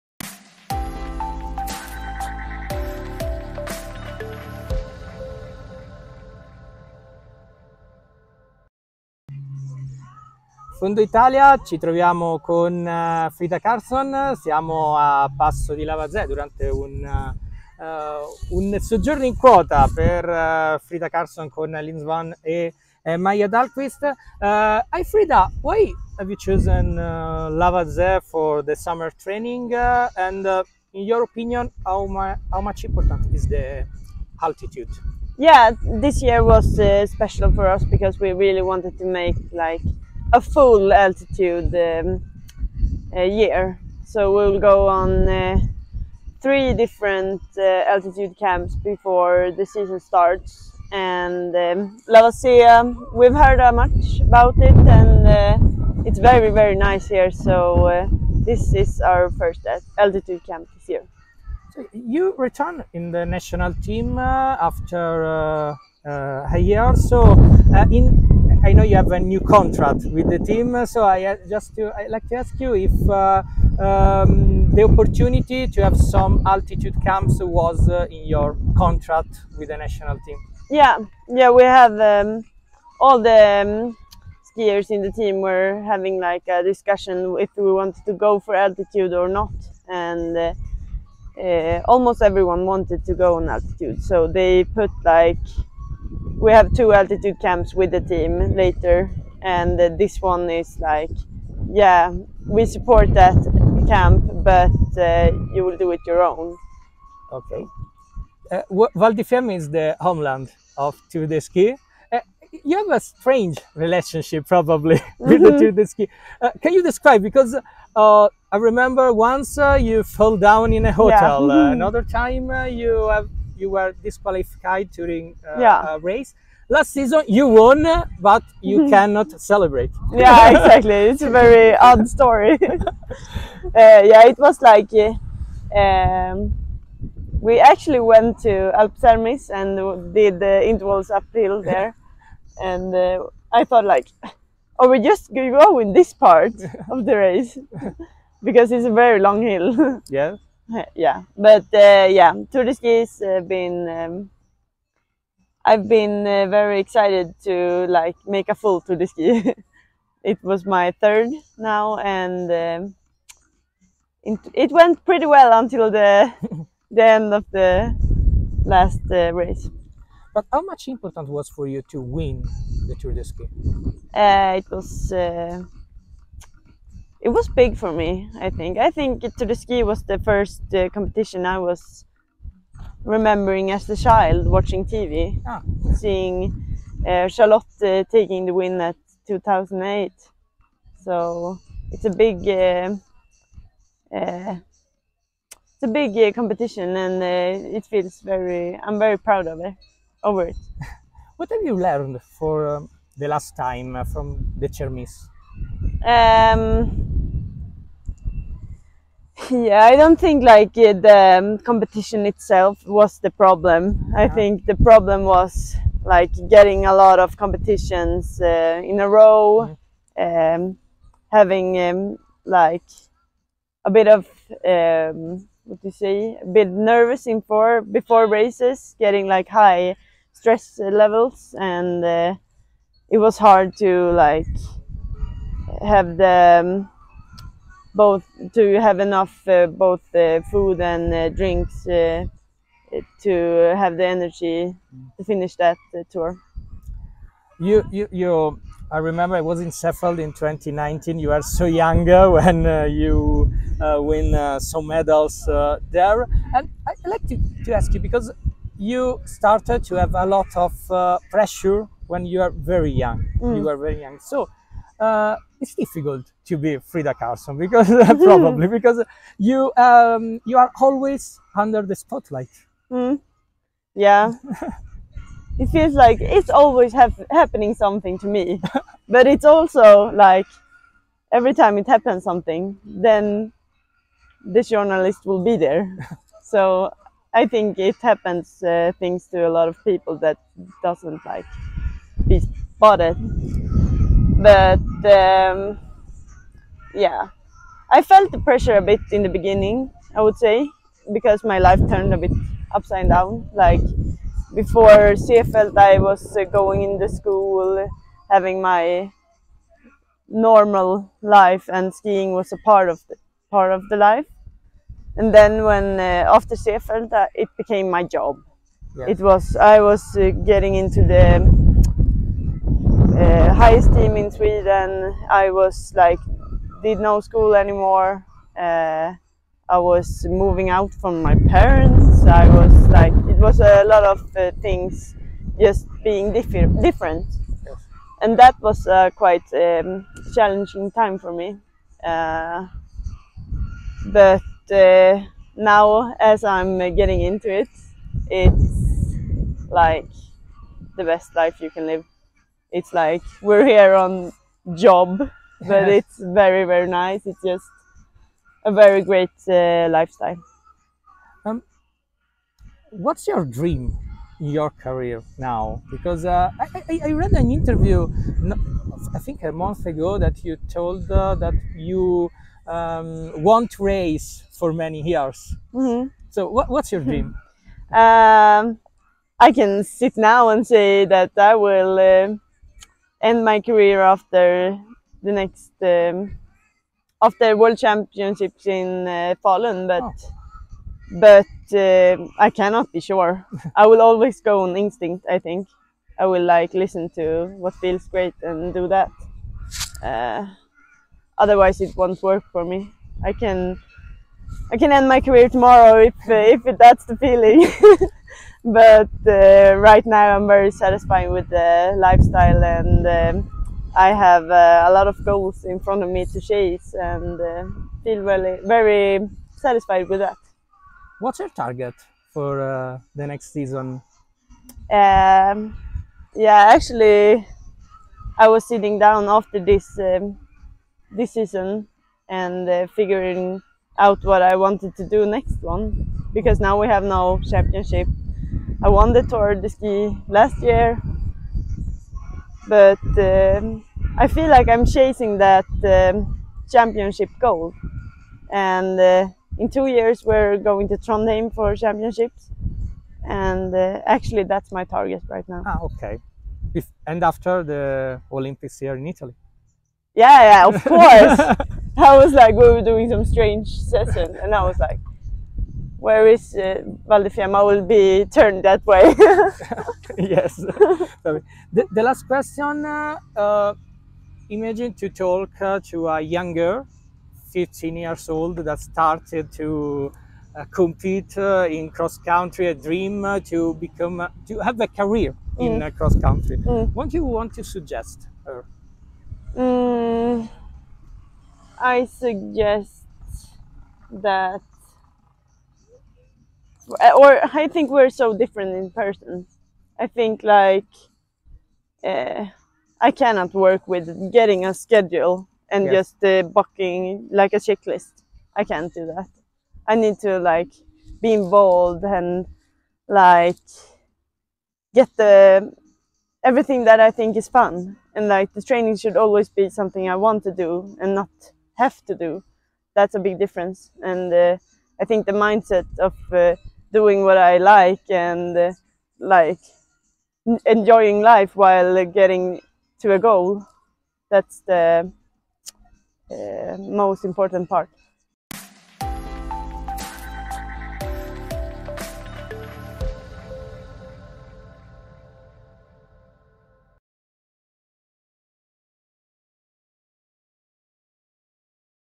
Cross Country Skiing - Interview to Frida Karlsson during her training camp in Val di Fiemme SUB ITA
La svedese, vincitrice dell'ultimo Tour de Ski, è tornata in Val di Fiemme, a Passo di Lavazè, dove, soggiornando al Dolomiti Apart & Rooms insieme a Linn Svahn e Maja Dahlqvist, si è fermata per ben tre settimane per immagazzinare un po' di allenamento in quota.